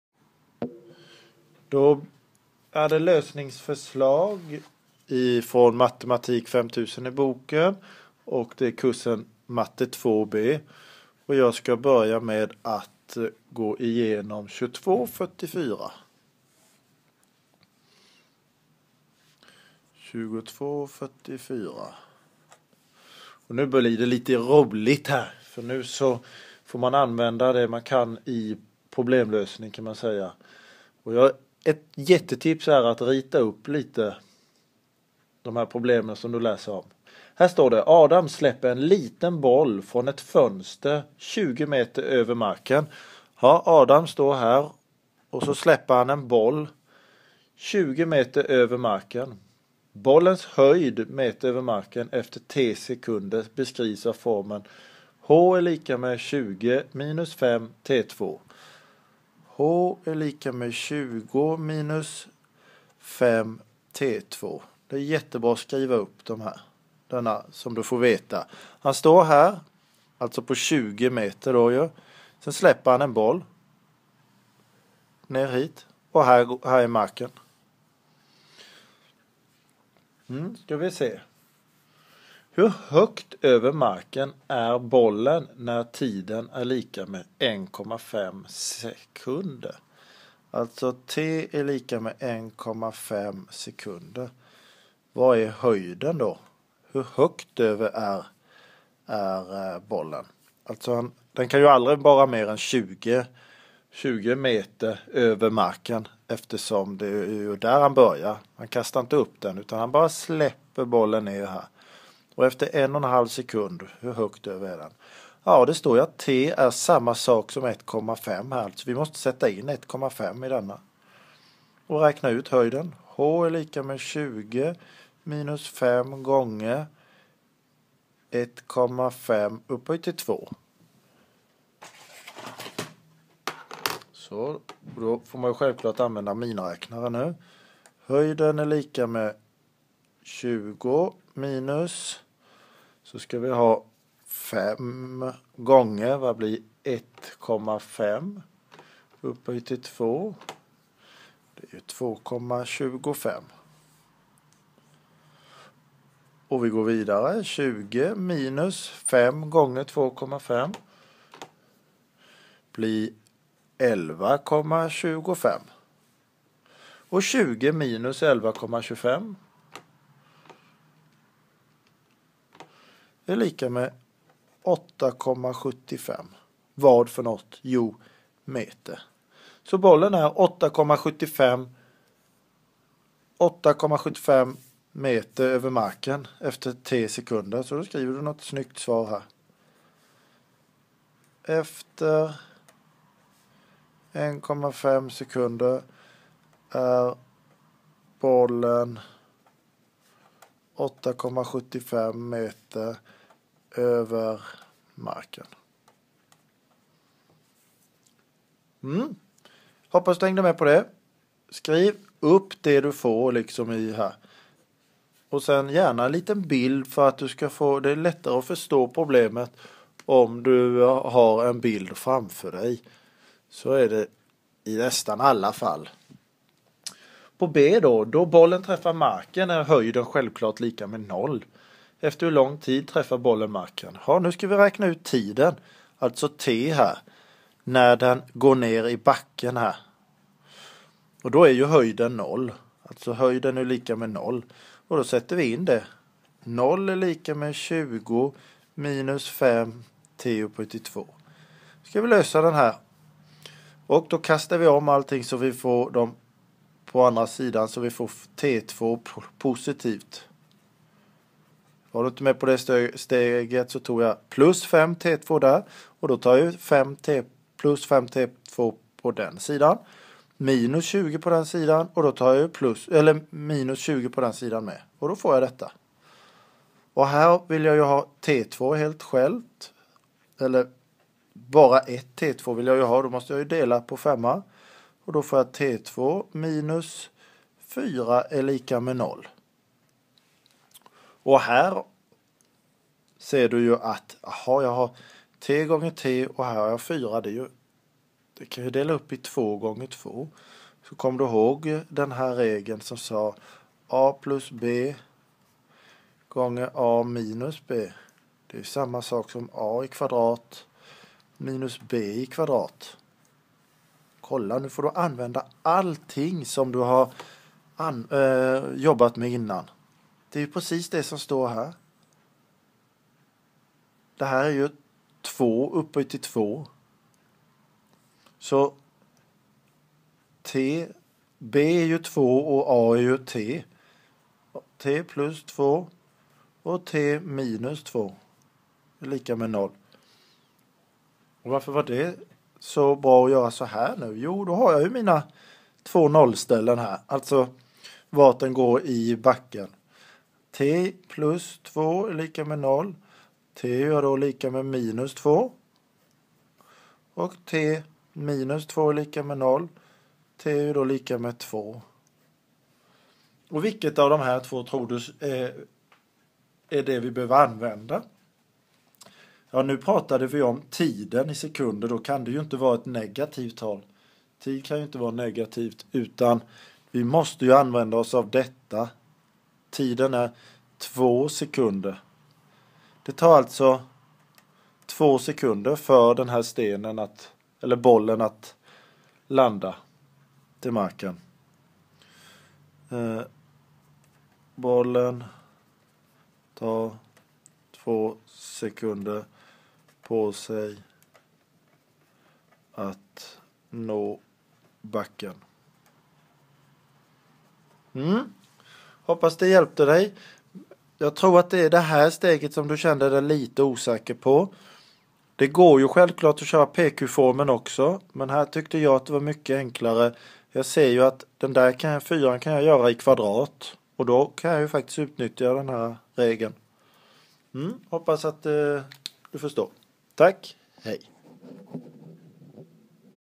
Dalles matte - Ma2b lösningsförslag kap 2 del 4
Gå gärna in på för att se fler inspelningar och här är alla avsnitt bättre organiserade så att du hittar det du söker lättare.

I detta avsnitt får du lösningsförslag på uppgift 2244 ur boken Matematik 5000 i kursen Ma2b.